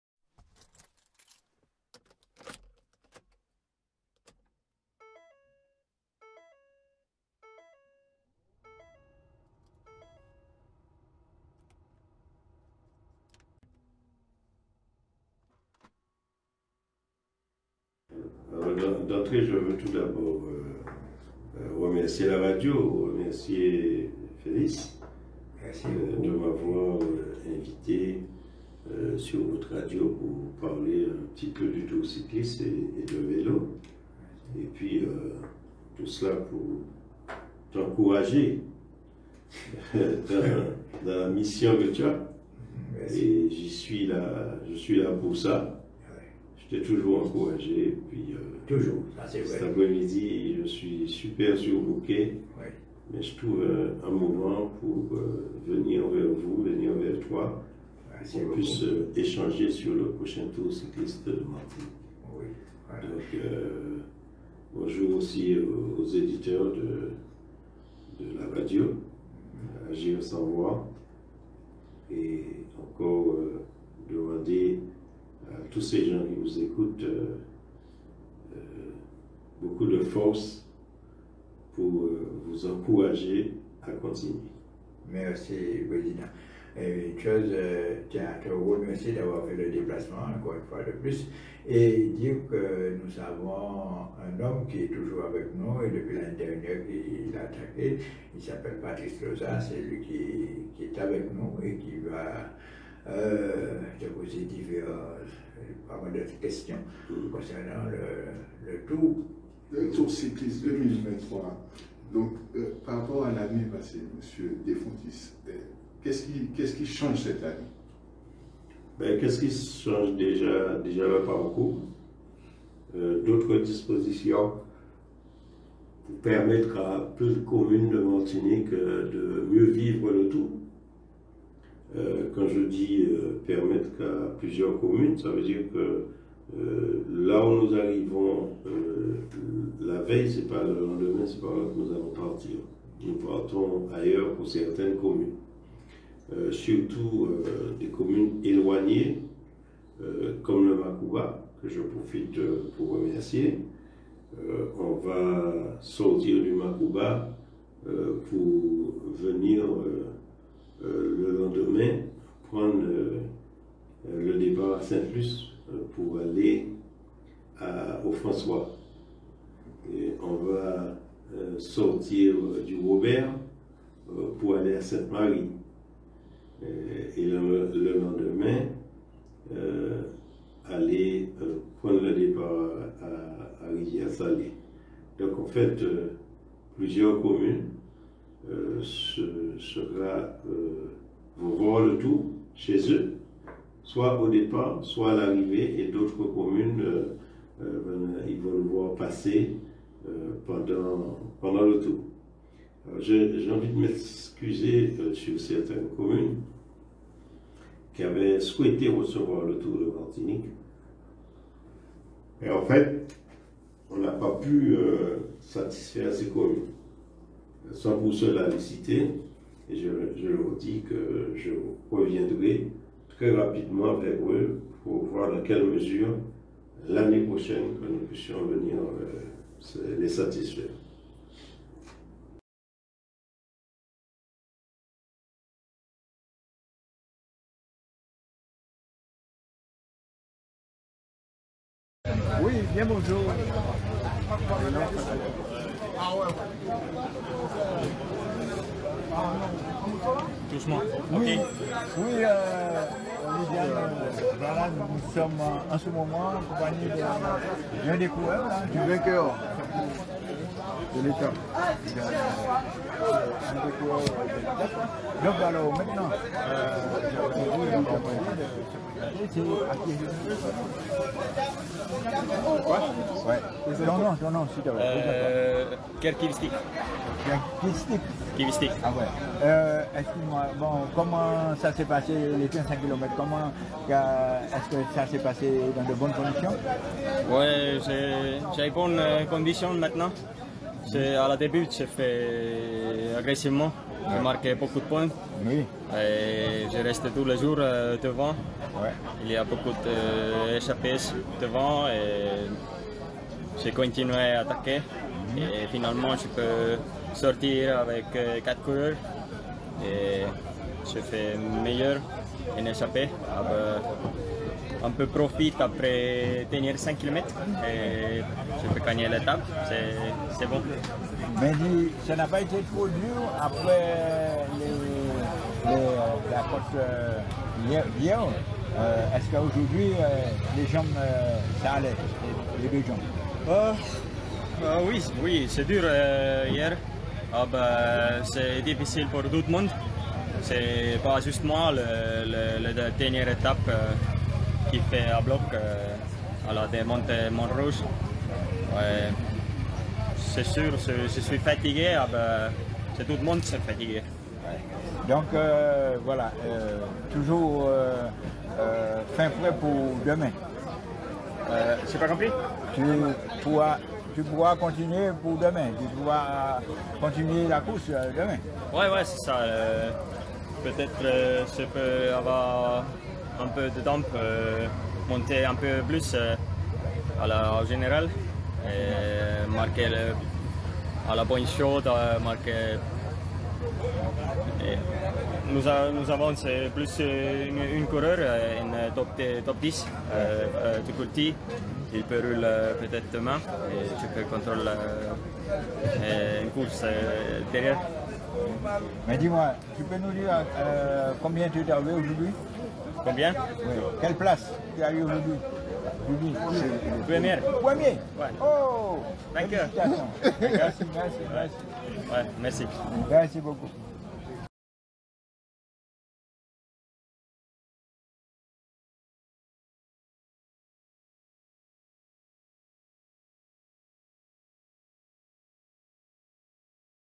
tour cycliste martinique 2023 parcours
| Découvrez plus d’infos sur l’agenda sportif

Les étapes
Samedi 8 juillet :
Saint-Joseph - Marin
Dimanche 9 juillet :
Marin - Fort-de-France
2e tronçon Fort-de-France - Fort-de-France (contre-la-montre)
Lundi 10 juillet :
Fort-de-France - Trinité
Mardi 11 juillet :
Rivière-Salée - Macouba
Mercredi 12 juillet :
Robert - Sainte-Marie
Jeudi 13 juillet :
Sainte-Luce - François
Vendredi 14 juillet :
François - Ducos
Samedi 15 juillet :
Dimanche 16 juillet :
Rivière-Pilote - Lamentin